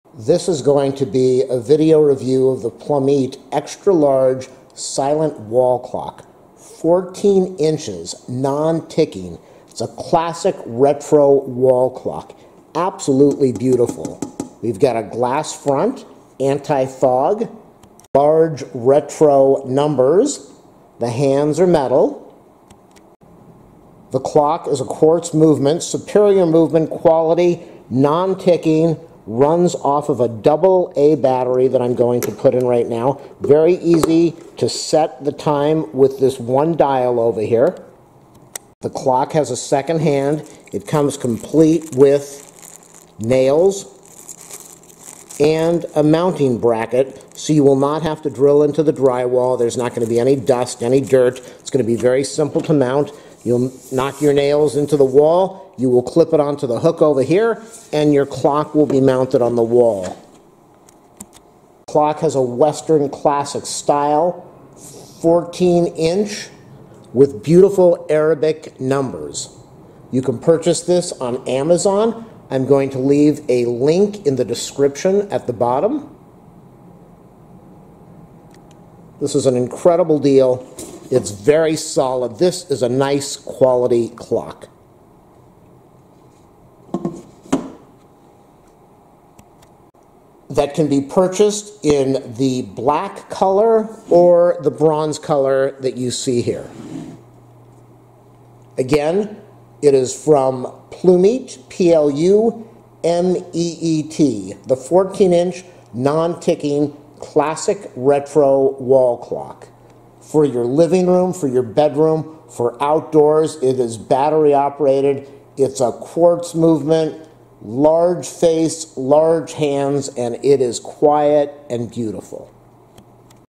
Plumeet Extra Large 14" Silent Wall Clock Non Ticking Classic Retro Wall Clock REVIEW
Plumeet Extra Large Silent Wall Clock, 14'' Non Ticking Classic Retro Wall Clock Decorative Living Room, Bedroom, Outdoors, Battery Operated Quartz Large Quiet Bronze Wall Clock (Metal Shell)